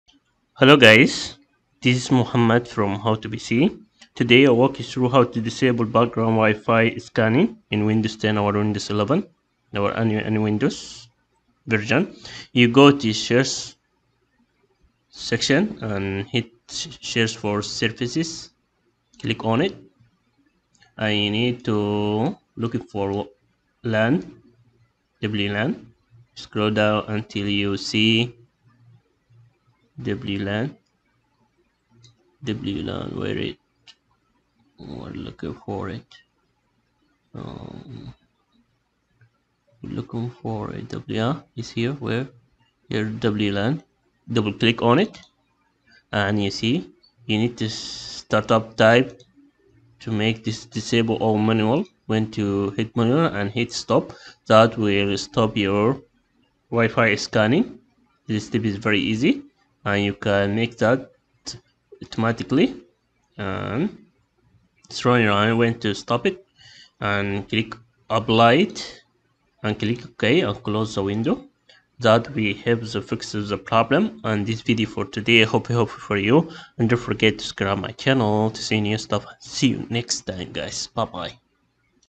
How to Disable Background Wifi Scanning in Windows 10 - Disable Wifi Background Scanning (2022)
Even when we choose to connect our PC to a home wireless network, Windows automatically scans for other wireless networks in range. Fortunately, there exists a setting that lets you disable background WiFi scanning in Windows 10.